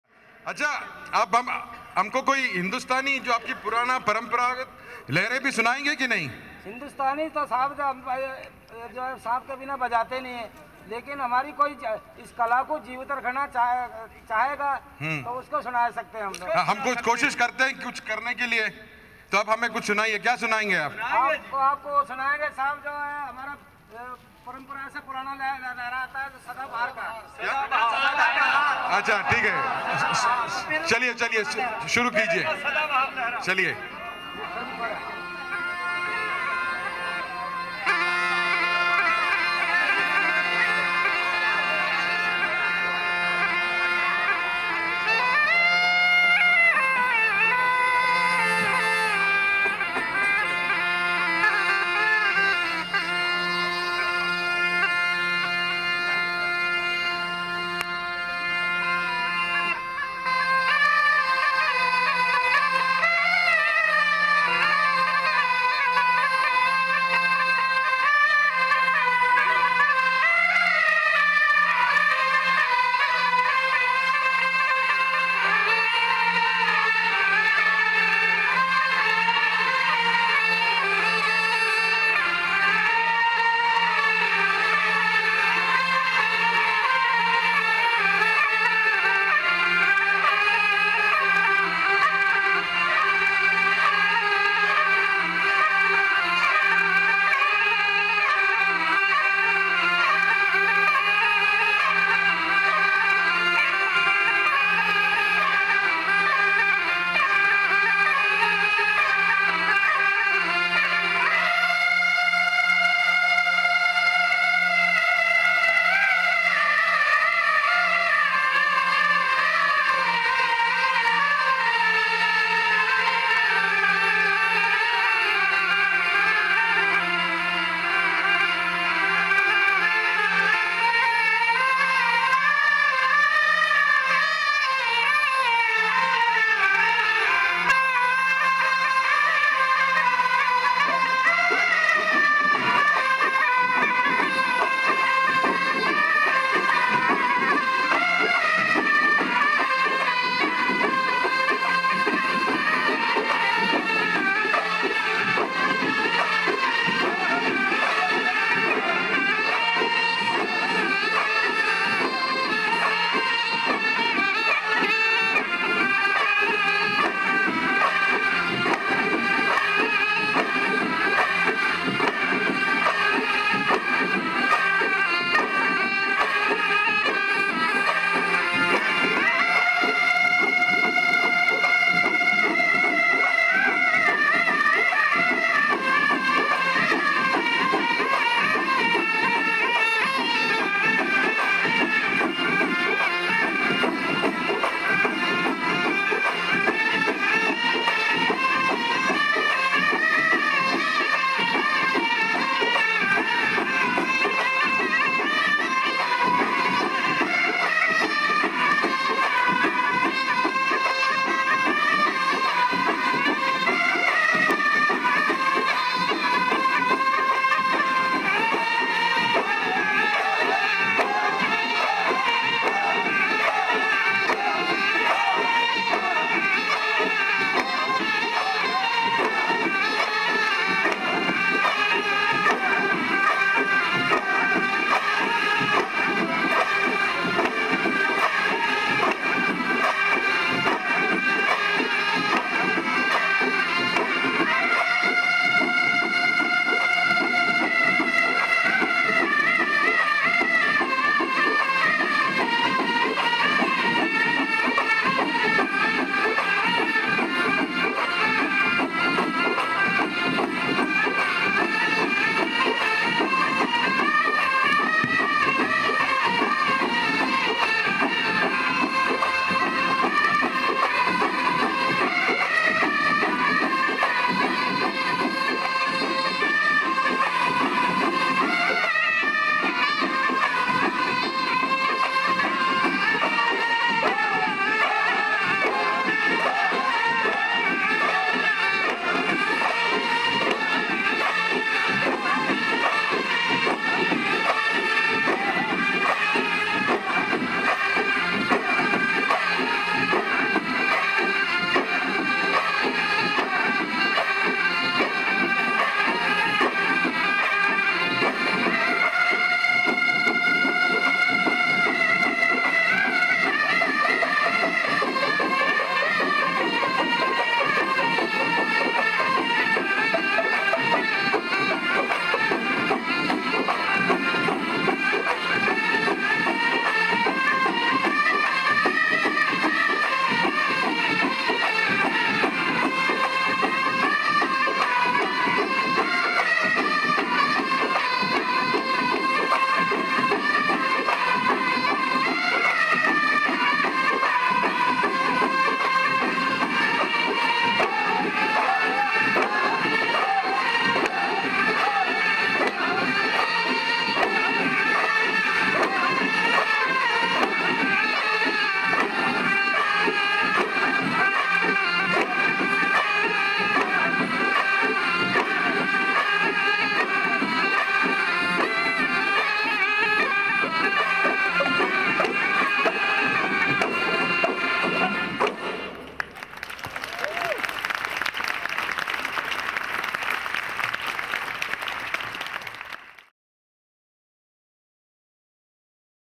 Snake charmers mass together for a rare musical performance
Snake charmer says "We do not play without a snake but if any Indian is interested in keeping this art alive then, of course, we will play."

An amazing musical congregation of the snake charmers showcasing the hidden charm of their music to the Indians. This evening of 'A Hundred Charmers' was organised by Friends of Snakes & Craft revival trust. This folk music concert was enabled through the talent of Jogi-Nath snake charmers community.